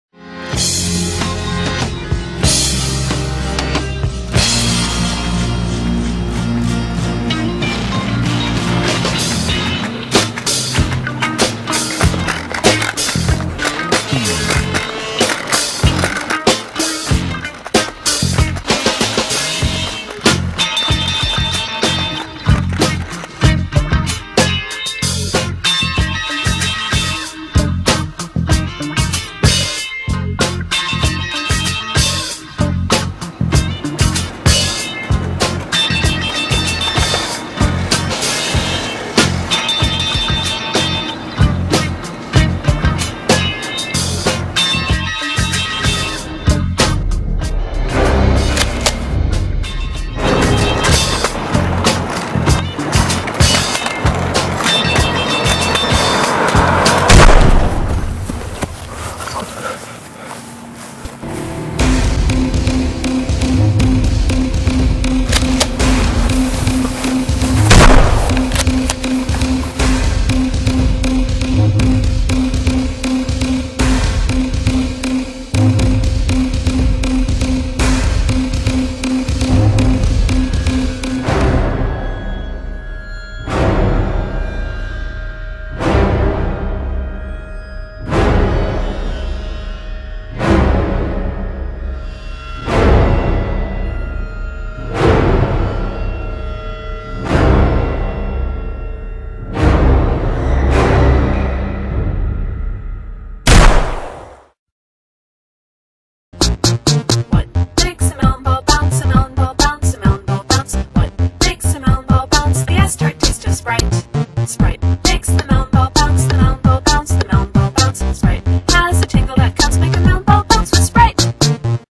GUNS and SHORT SHORTS
This is mindless filming at its best. No story, No script, just us filming ourselves being idiots. It was fun and goofy